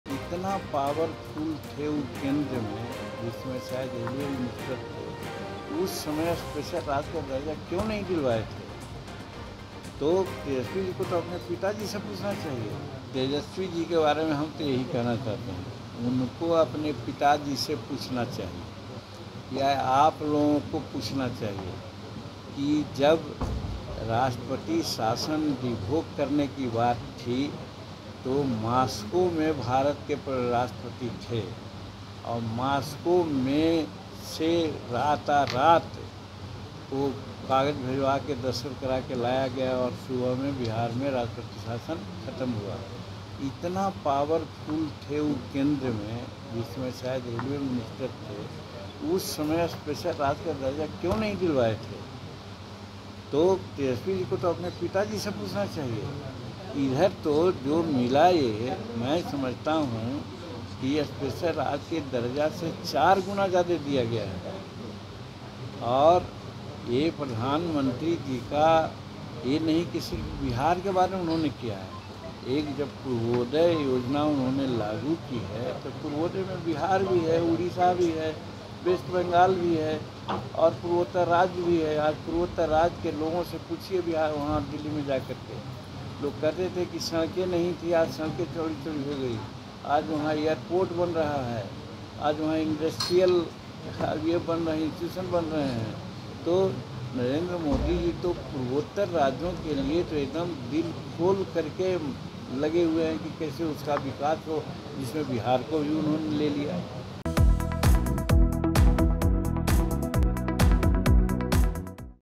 Jitan Ram Manjhi ने Tejashwi Yadav की लगा दी क्लास, Lalu Yadav से पूछिए ये सवाल| Bihar Tak
बिहार को विशेष राज्य का दर्जा नहीं मिलने पर विपक्ष लगातार नीतीश सरकार को घेर रहा है..जिसपर जवाब देते हुए जीतन राम मांझी तेजस्वी पर निशाना साधा है..जीतन राम मांझी ने कहा कि तेजस्वी को अपने पिताजी से पूछना चाहिए कि बिहार को विशेष दर्जा क्यों नहीं दिला सके..